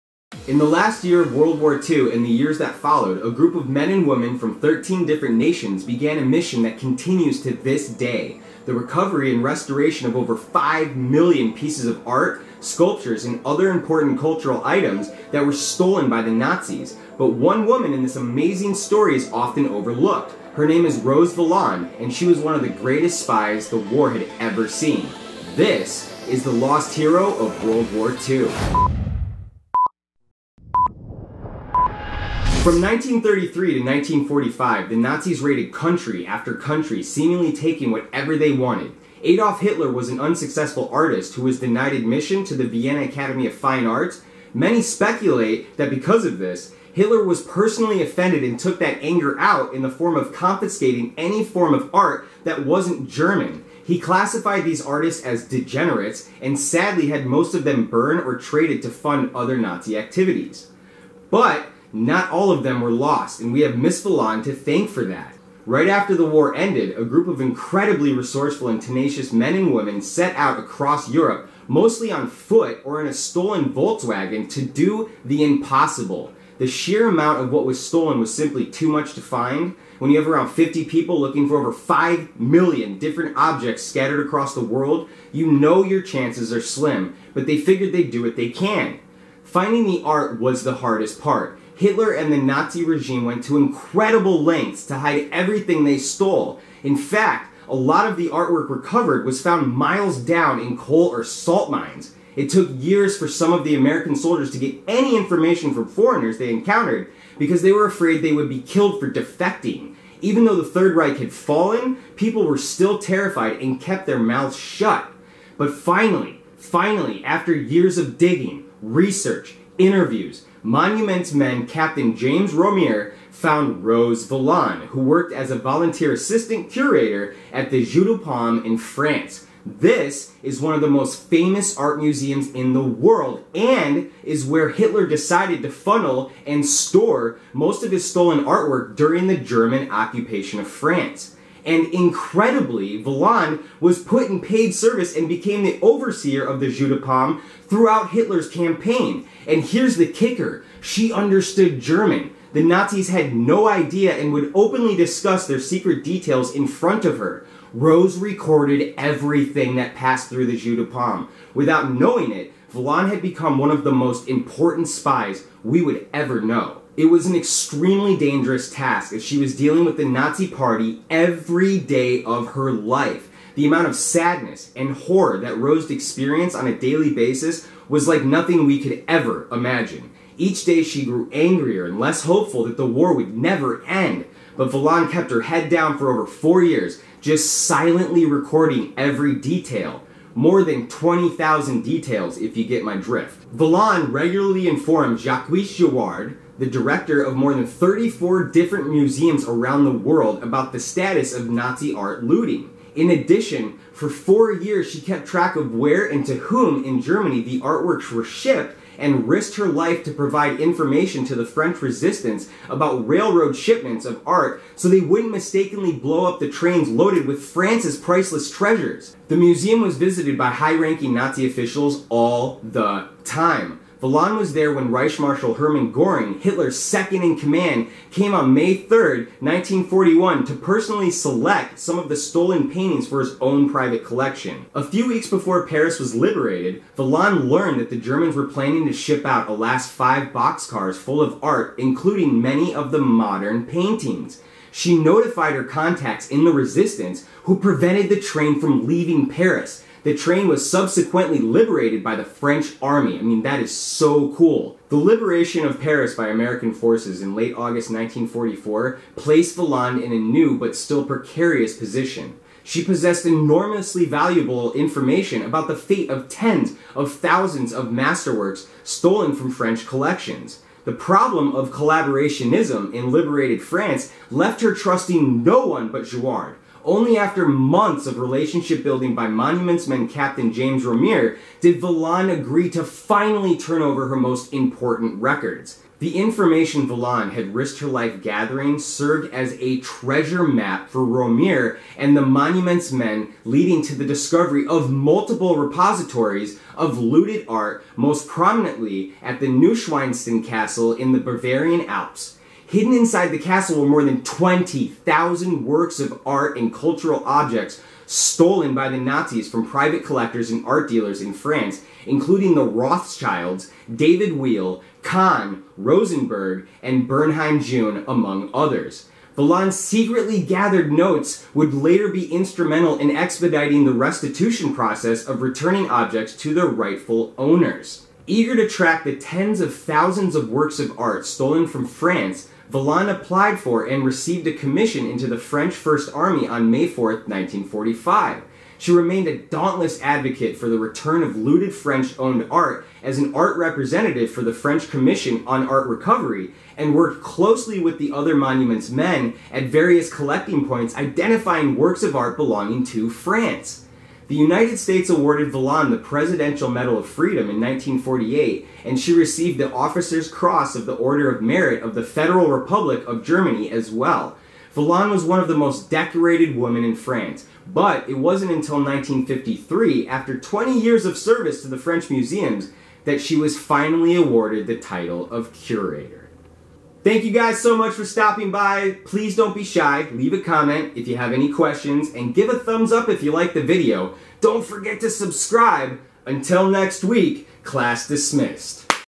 THE LOST HERO OF WORLD WAR II (Rose Valland & The Recovery Of 60,000 Masterpieces)
Rose Valland Is Considered To Be One Of The Greatest Spies Of World War II... This Is Her Story